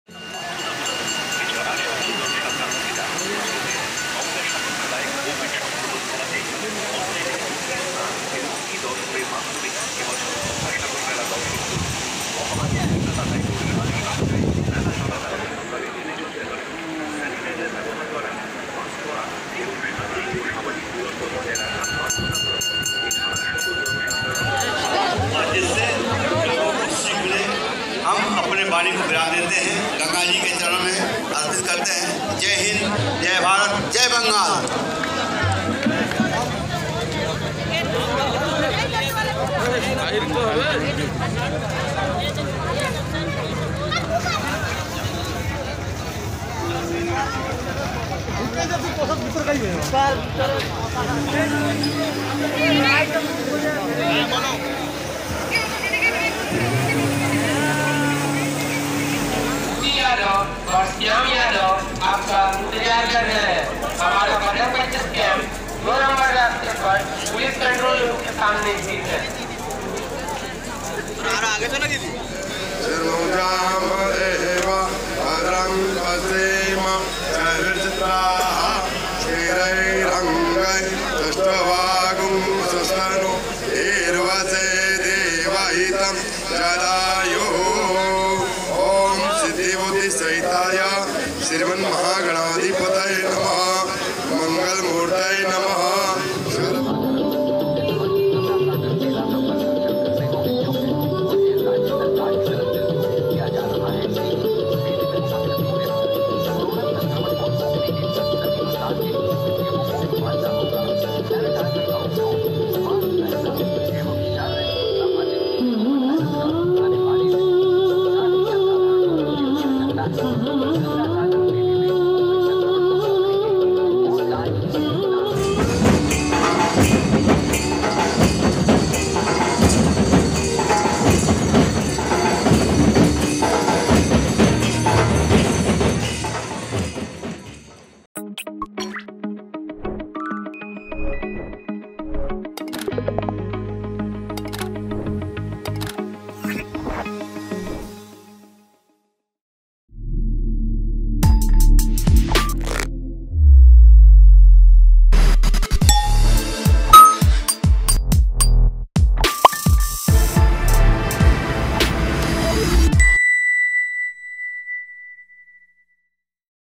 Lakhs of Devotees Throng Gangasagar Mela Amid Rise in COVID Cases in West Bengal | The Quint
Even as West Bengal reported 22,645 new cases of COVID-19 on Friday, 14 January, with a positivity rate of 31.14 percent, lakhs of devotees thronged the Gangasagar Mela in the state, to take a holy dip at the point where the Ganges joins the Bay of Bengal.